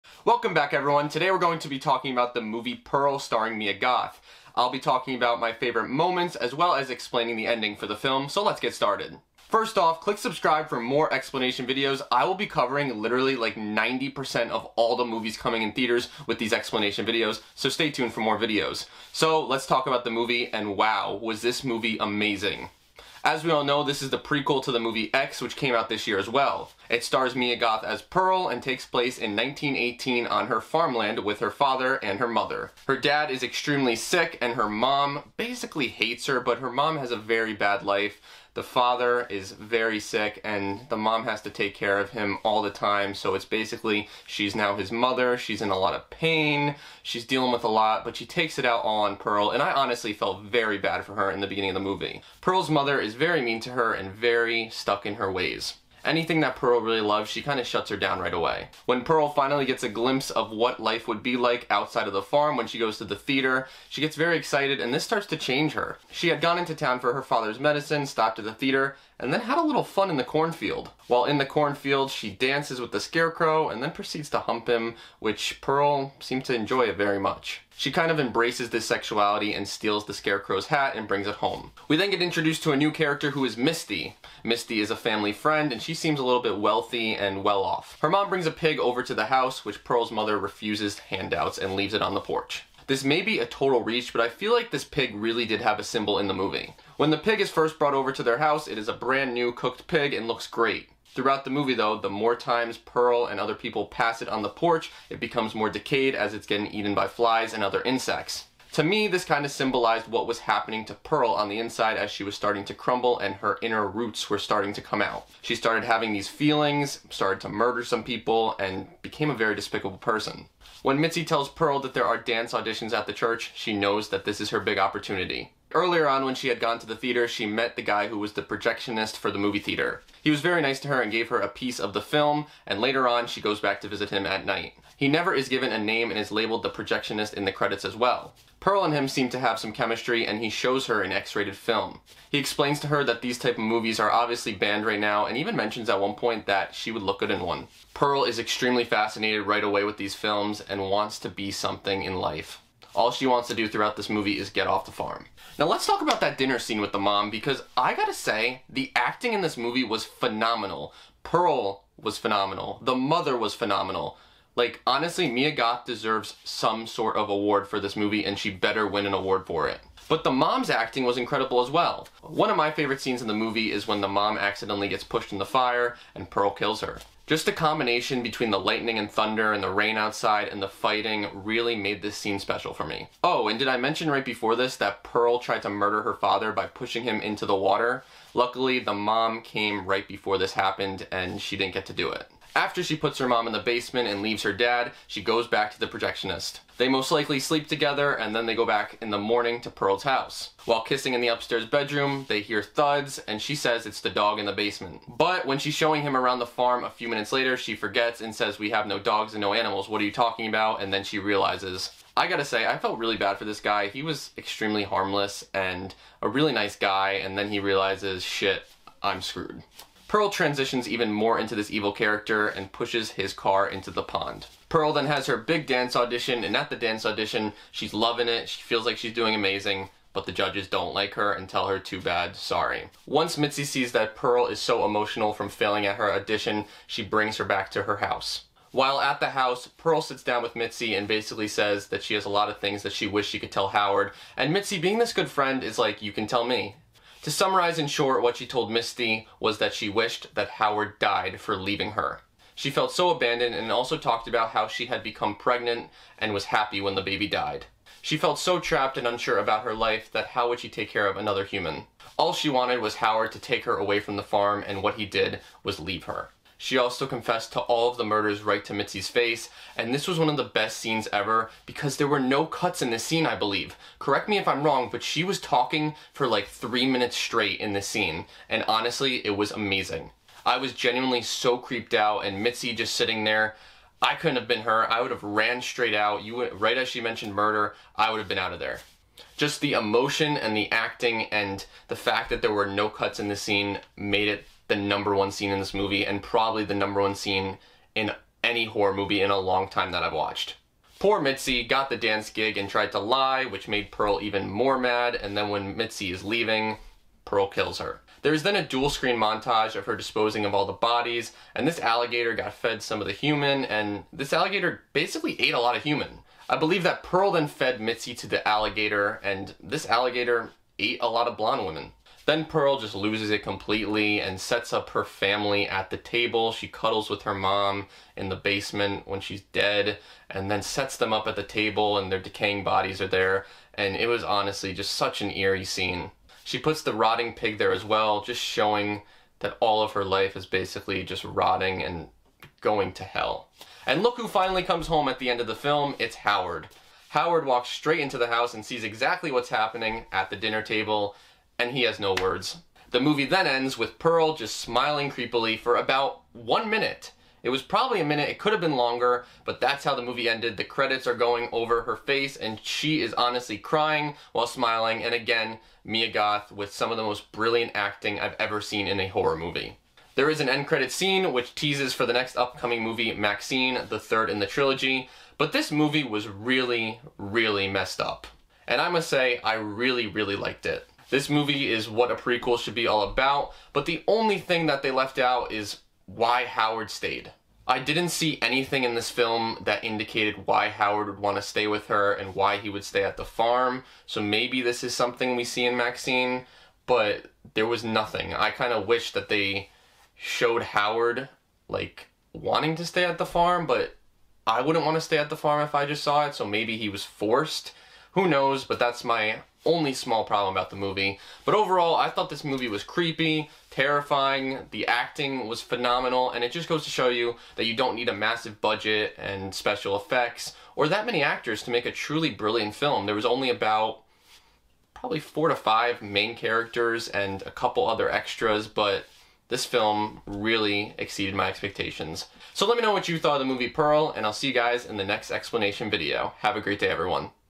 Pearl Ending Explained (Mia Goth Pearl Horror Movie Breakdown)
Devin explains the ending of the movie Pearl. He gives a Pearl Movie Review, Pearl Explantation, talks about his favorite Pearl Movie Scenes as well as his favorite characters. Mia Goth is amazing in this film.

Plot Summary: Pearl (also marketed and known as variations of Pearl: An X-traordinary Origin Story) is a 2022 American slasher film directed by Ti West, and co-written by West and Mia Goth, who reprises her role as the titular character. David Corenswet, Tandi Wright, Matthew Sunderland, and Emma Jenkins-Purro feature in the supporting cast. A co-production of A24 and Little Lamb Productions, it is a prequel to X (2022) and serves as an origin story for the titular villain.